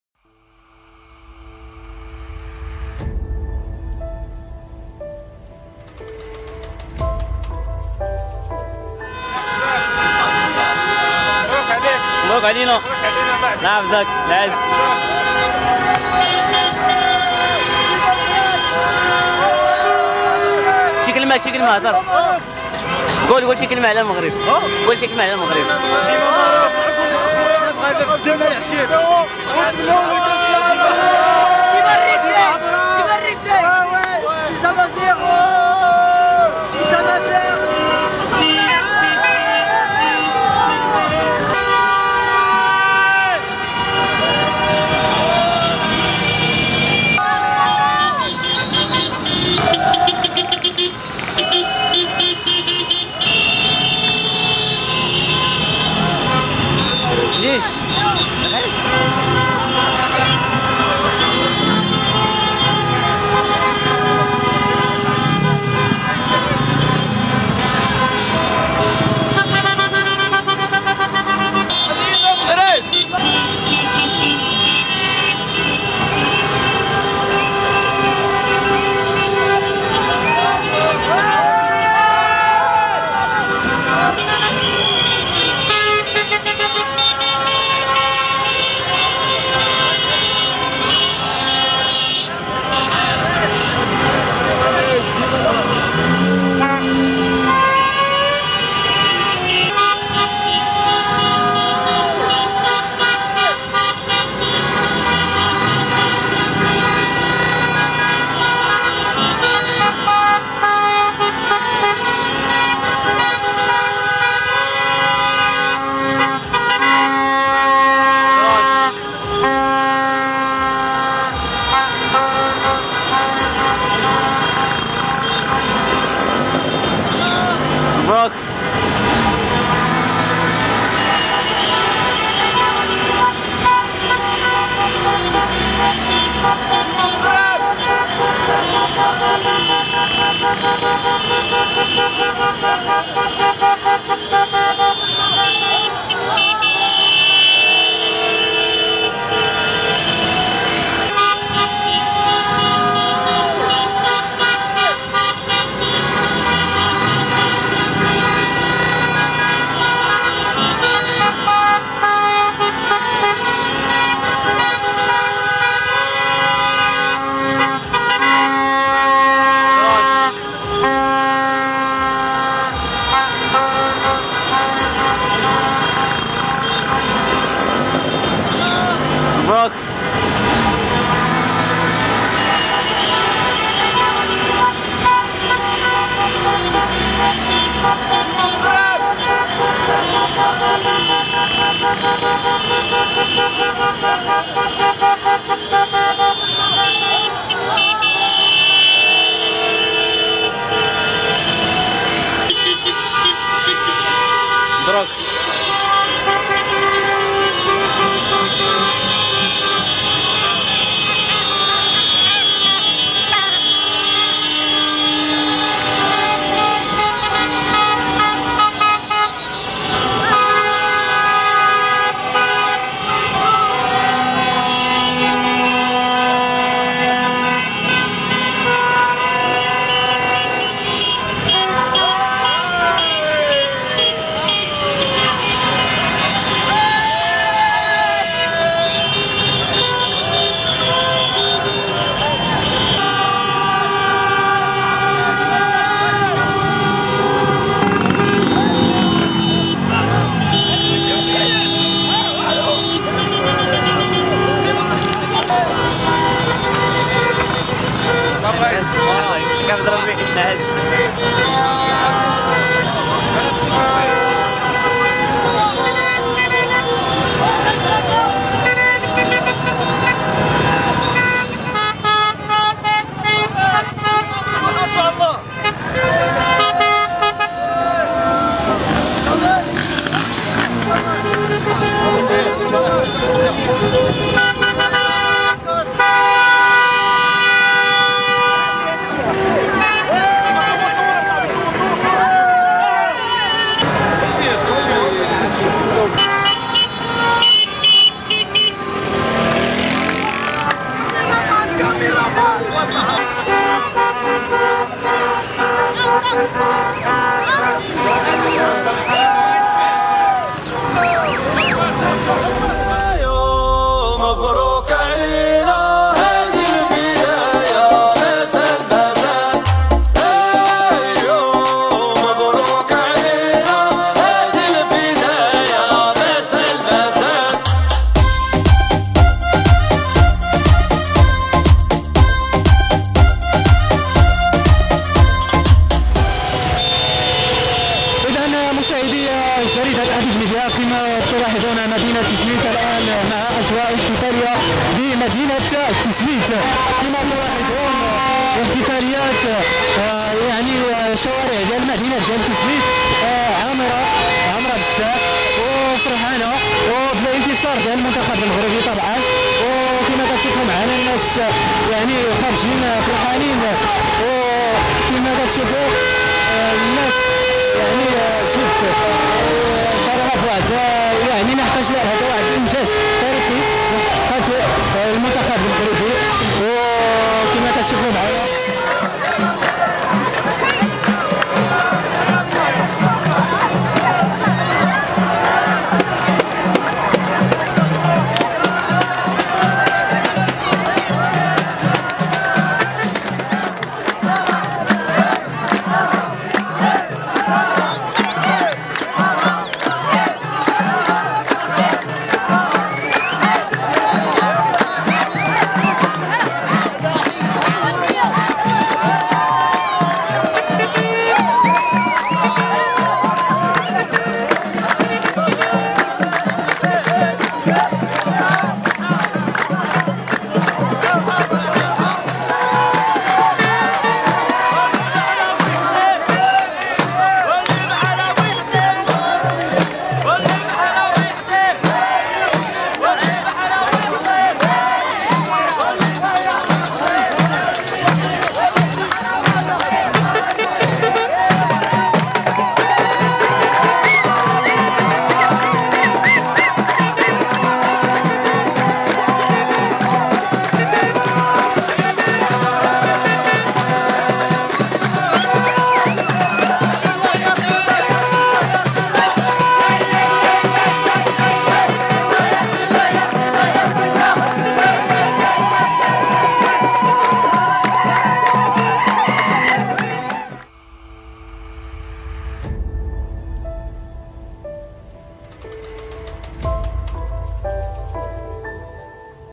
من قلب تيزنيت ...فرحة المغاربة بالانتصار على البرتغال والتأهل إلى النصف النهائي ..
زوروا  موقعنا للاطلاع على آخر الأخبار